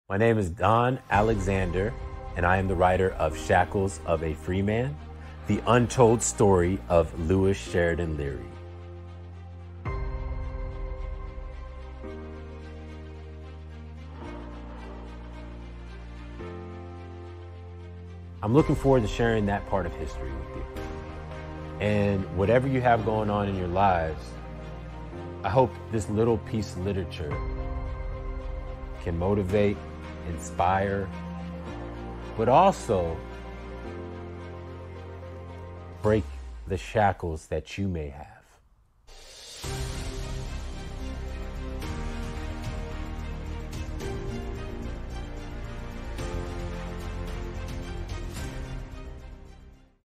Discovering "Shackles of a Freeman"
Step back in time and join Lewis Leary on his journey to fight against the injustices of slavery in 1850s America, in 'Shackles of a Freeman,' a gripping and thought-provoking historical fiction novel shedding light on the Anti-Slavery Movement and the bravery of those who fought against it.